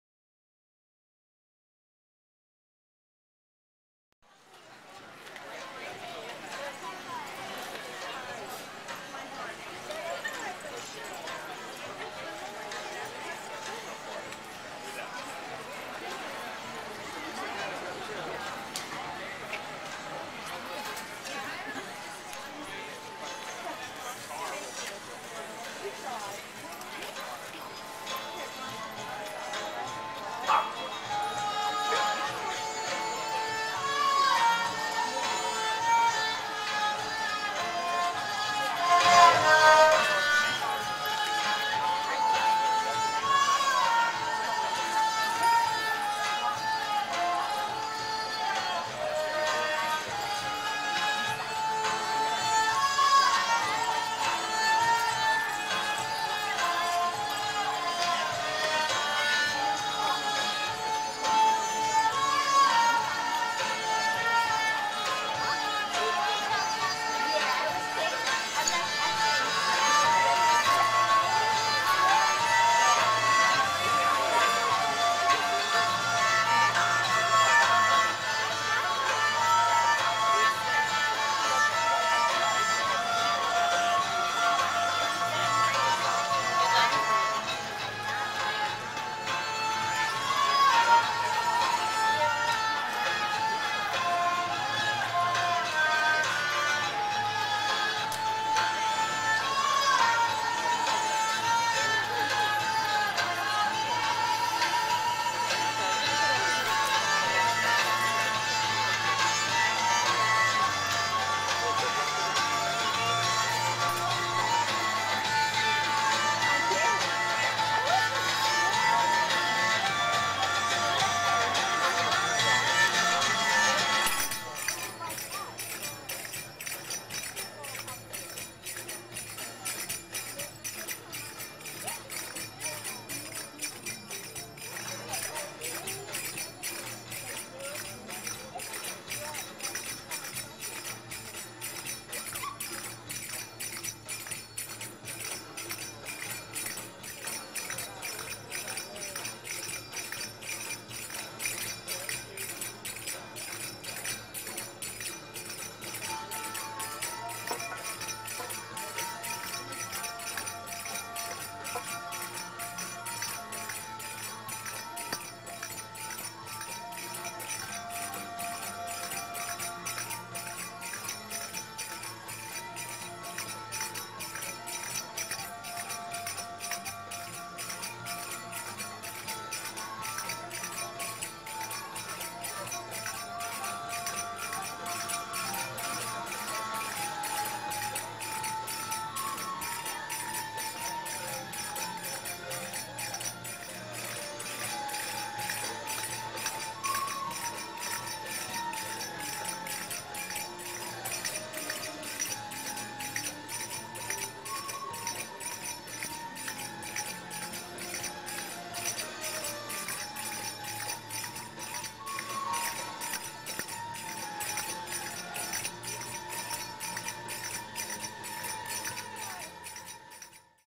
Tribe Hamsa @ Van Duzer St Fair (2/3)
Tribe Hamsa performs American Tribal Style Belly Dance at the Van Duzer Days street fair, Staten Island, NY.

ATS is a modern dance form created in San Francisco by Carolena Nericcio, director of Fat Chance Belly Dance It combines traditional belly dance with modified movements inspired by the folkloric and tribal dances of cultures from India, the Middle East, North Africa and Spain.

ATS dancers use these movements, along with a series of non-verbal cues and step combinations, to create group performances that can look carefully choreographed but are actually completely improvised.